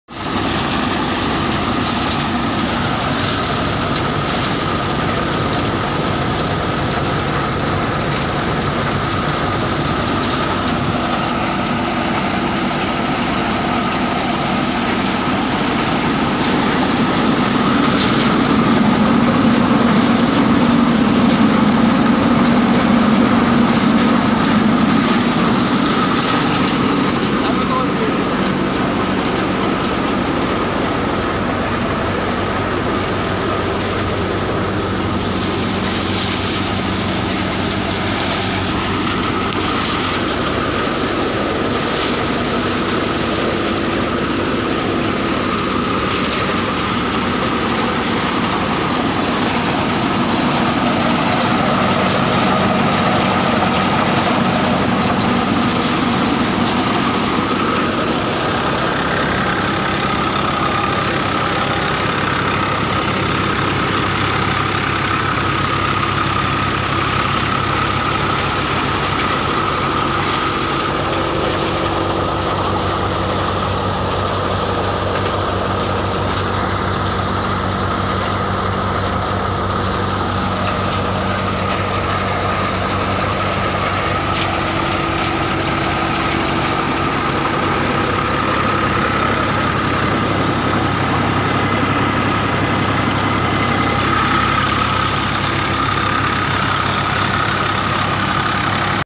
AM Combine Thresher threshing mung bean (9828504082,9982751587)
AM मिनी कंबाइन थ्रेशर से शानदार मूंग निकालते हुए किसान